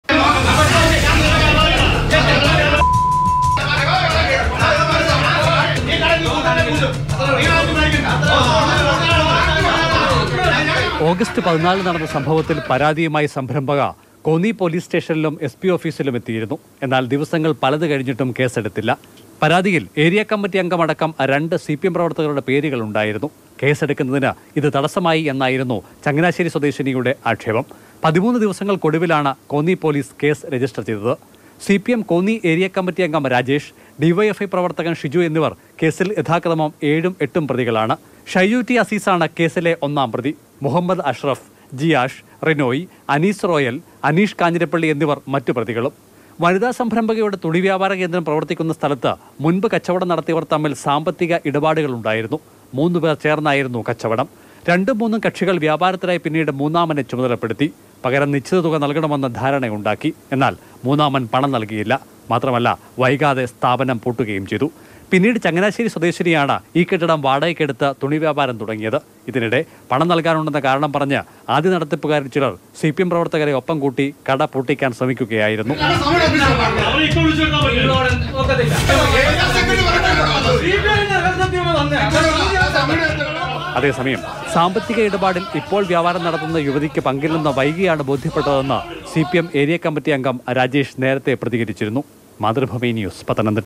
വനിതാ സംരംഭകയുടെ സ്ഥാപനം പൂട്ടിക്കാന്‍ ശ്രമം; CPM ഏരിയ കമ്മിറ്റി അംഗത്തിനെതിരെ കേസെടുത്തു
മാറുന്ന ലോകത്ത് കൂടുതൽ മാറ്റങ്ങളോടെ. പുതിയ വേഗത്തിൽ. പുതിയ ലുക്കിൽ.
മാതൃഭൂമി ന്യൂസ്.

Happy viewing!